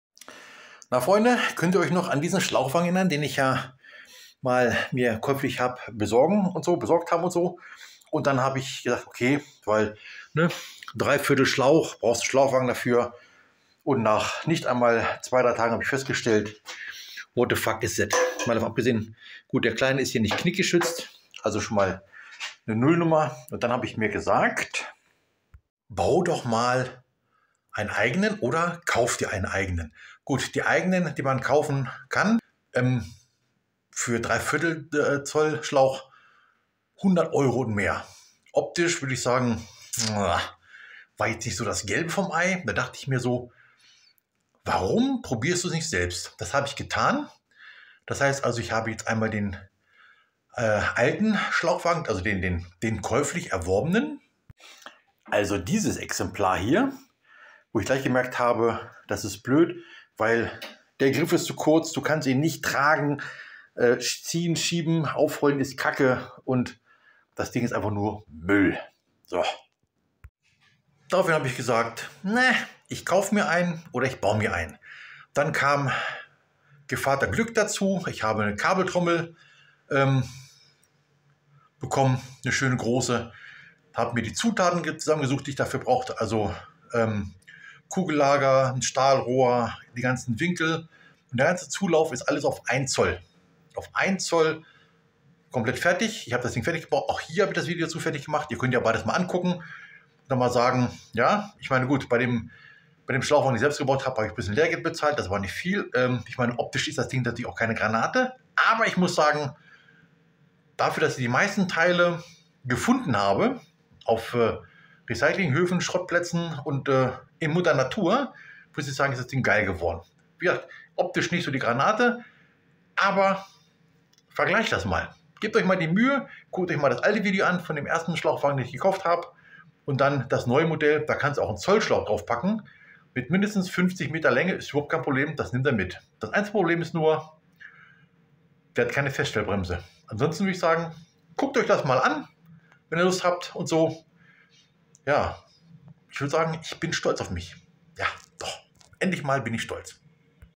Schlauchwagen vs Schlauchwagen
Habe mir einen Schlauchwagen gekauft. Nun ja, Reden wir nicht darüber.
Link oben rechts.
Alternativ, einen bauen! Ok, ist nicht der schönste, aber 1 " mit 40 metern püasen da locker drauf.
Link oben rechts
Hier der kleine dezente Hinweis, was wo ist.